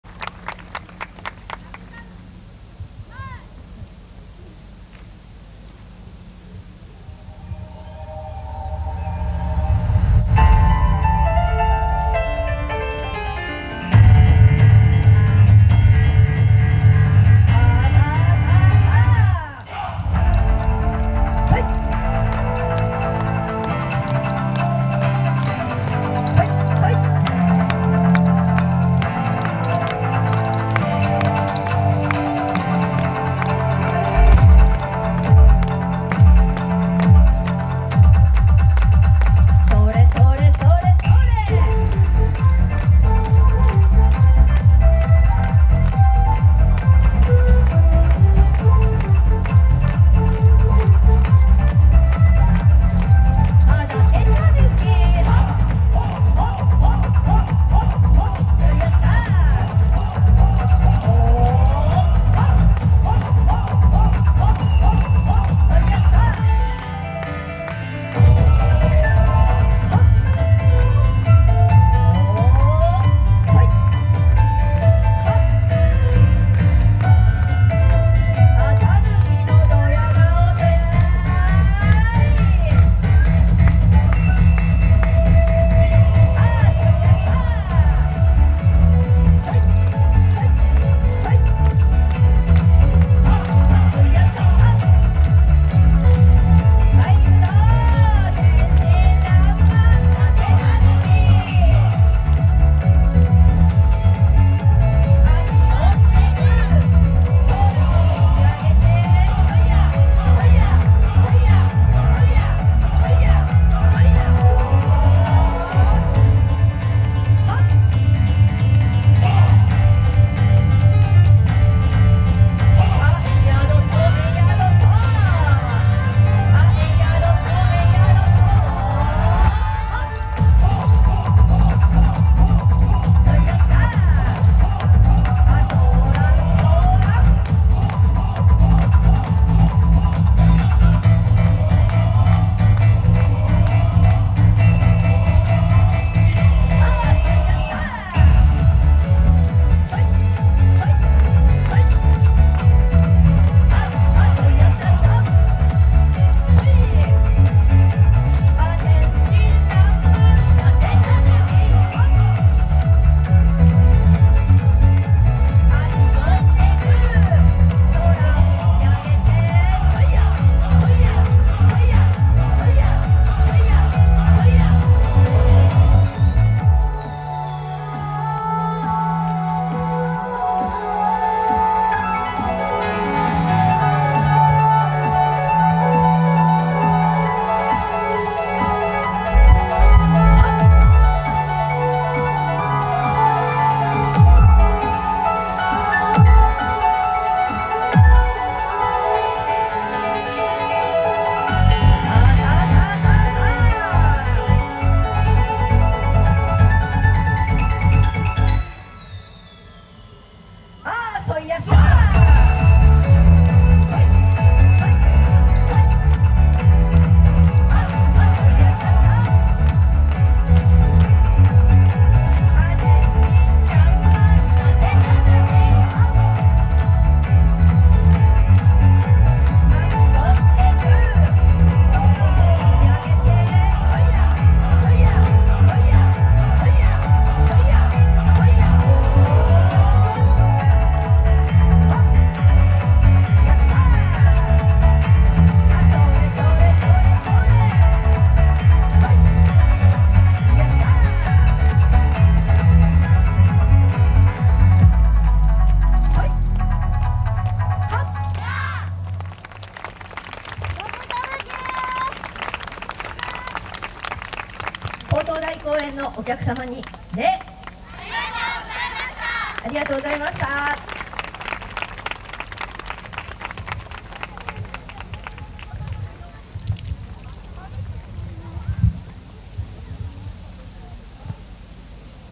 伊達たぬき 📍仙台みちのくYOSAKOIまつり (勾当台公園会場) 2024-10-12T19:06
2024年10月12日(土)、宮城県仙台市で開催された「仙台みちのくYOSAKOIまつり」(勾当台公園会場)での「伊達たぬき」さんの演舞です。